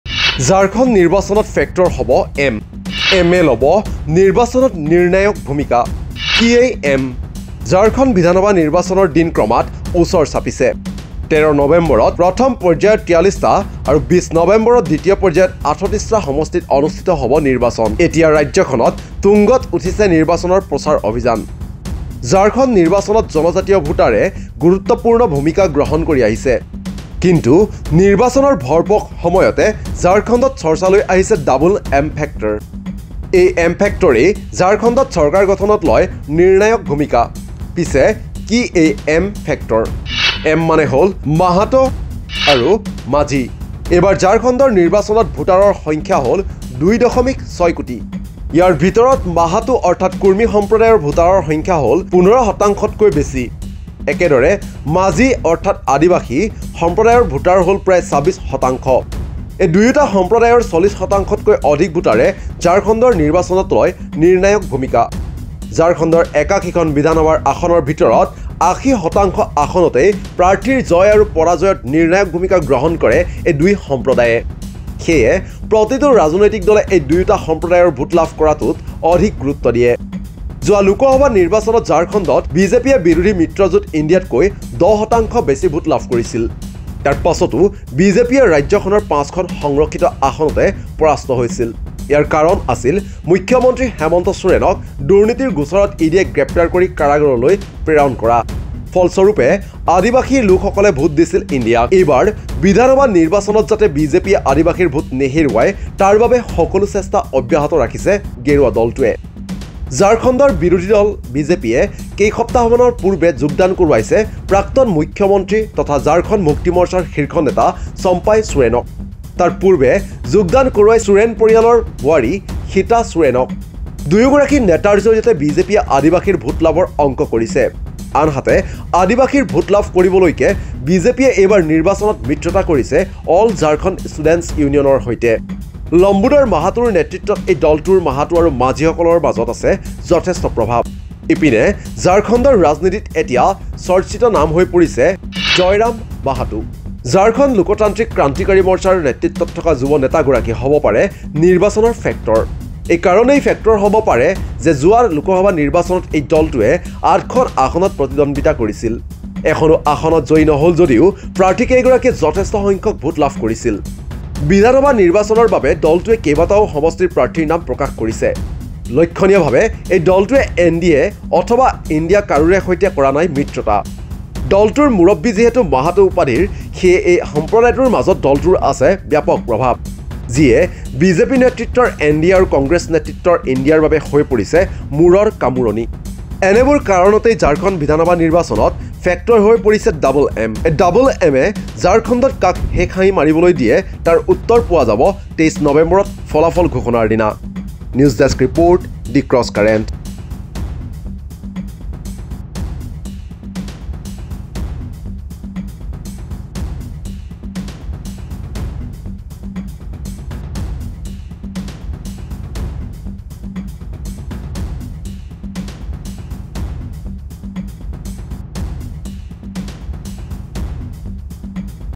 ঝাৰখণ্ড নিৰ্বাচনত ফেক্টৰ হ’ব ‘M’
ঝাৰখণ্ড নিৰ্বাচনত ফেক্টৰ হ’ব ‘M’। ‘M’-এ ল’ব নিৰ্বাচনত নিৰ্ণায়ক ভূমিকা। কি এই ‘M’?